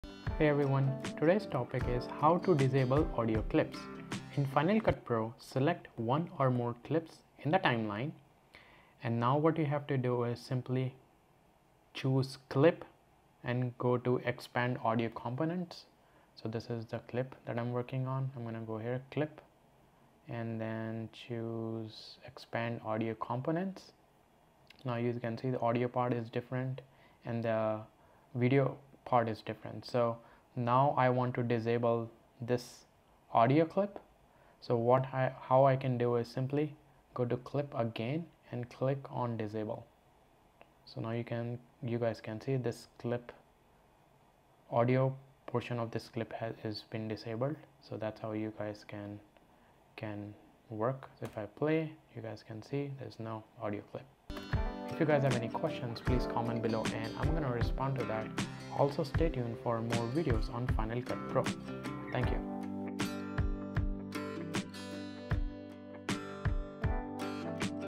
How To Disable Audio Clips In Final Cut Pro
1. In Final Cut Pro, select one or more clips in the timeline.
Note: If the selected clips contain both video and audio, and you want to mute the audio only, choose Clip - Expand Audio or Clip - Expand Audio Components, then select the audio portion only (or drag to select a range).
2. Choose Clip - Disable (or press V).
Disabled clips are both invisible and silent and do not appear in the viewer or in any output. The filmstrips of disabled clips appear dimmed in the timeline.